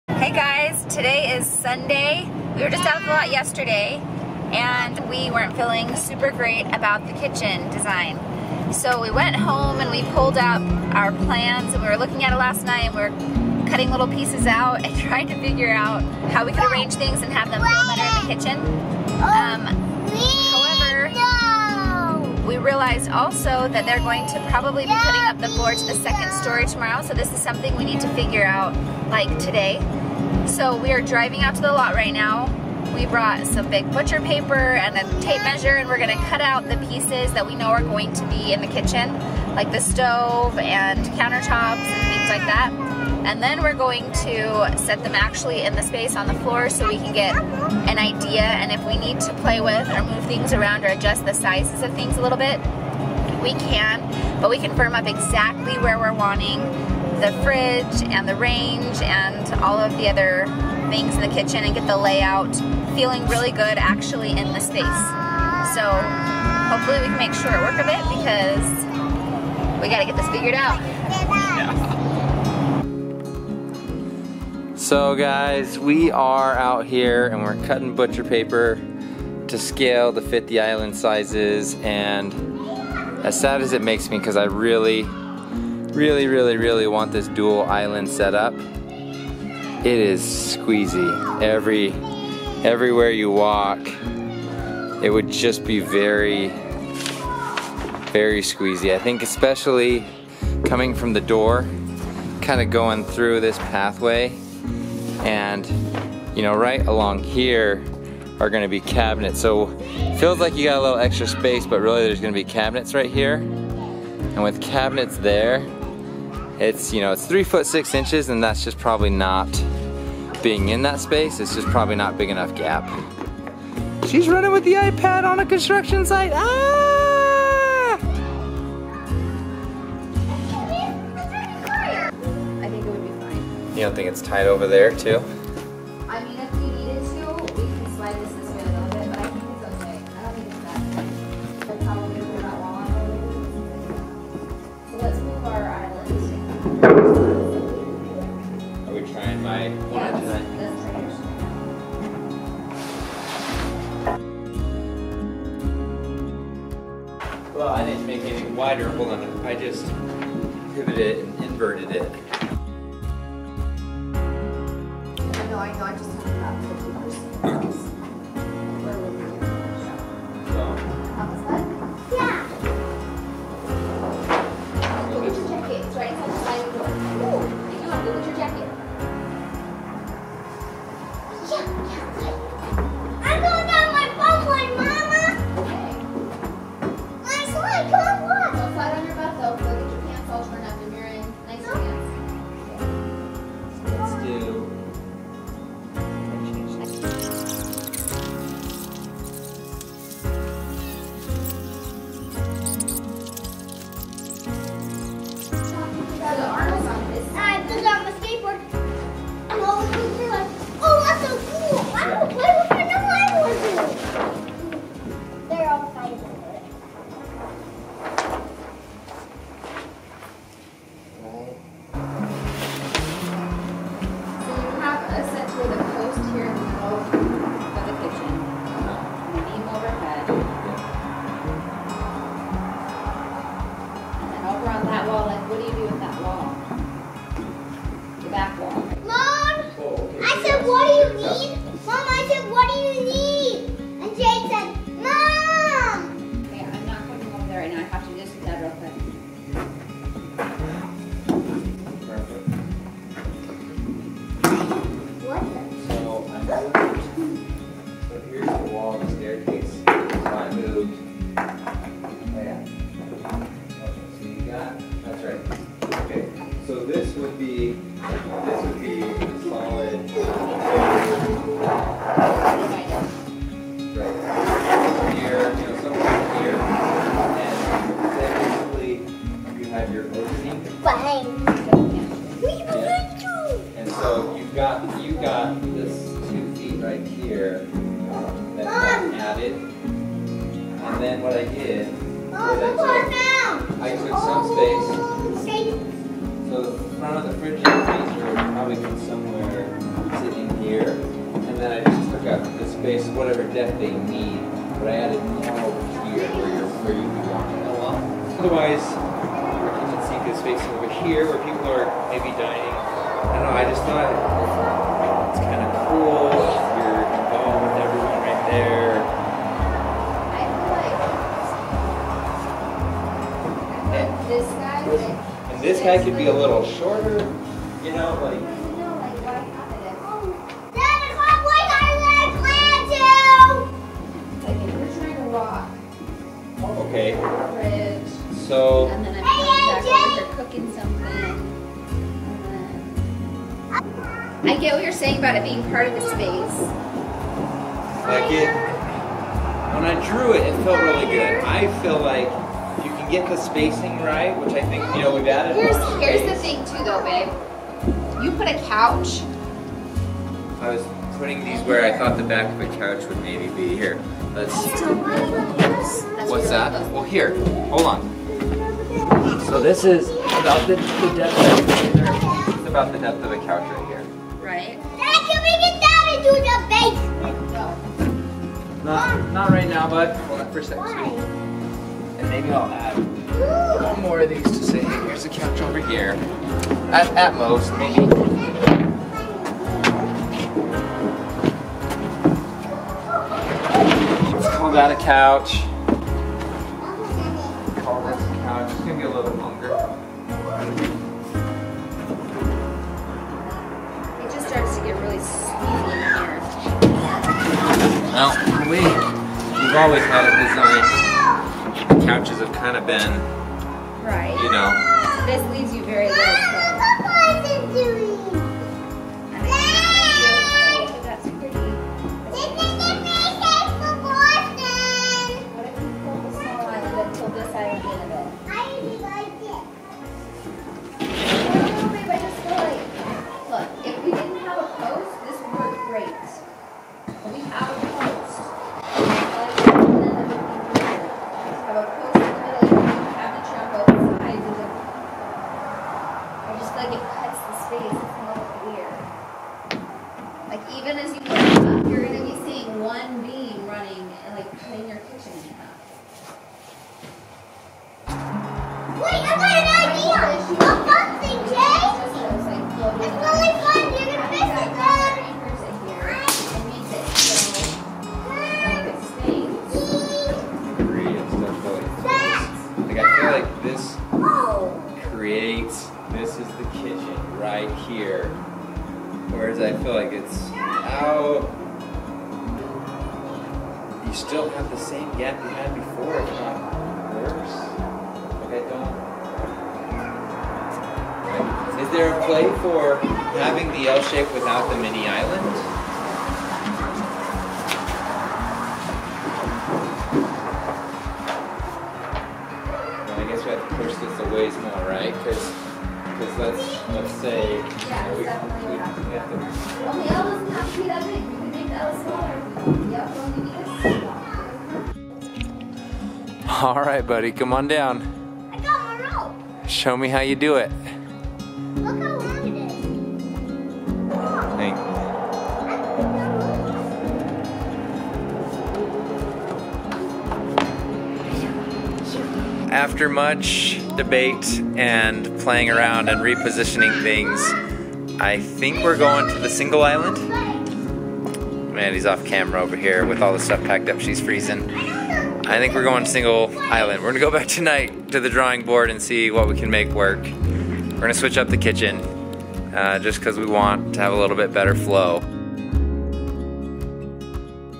Questioning Our Kitchen | Dirt to Dream Episode 29
After visiting our house, we're still struggling with the layout of our kitchen. It's probably one of the most important spaces in the home, and unfortunately happens to be one of the spaces we're least sure about. Help! How many islands should we include - 1 or 2? Which direction should the kitchen island face - the living space or the dining space? The TV/fireplace or the back windows? Where should the kitchen sink go? How about the range? The refrigerator and freezer? We probably need to hire a kitchen designer. Any recommendations?

*highfive*
(leave any food Emoji in the comments if you read this far)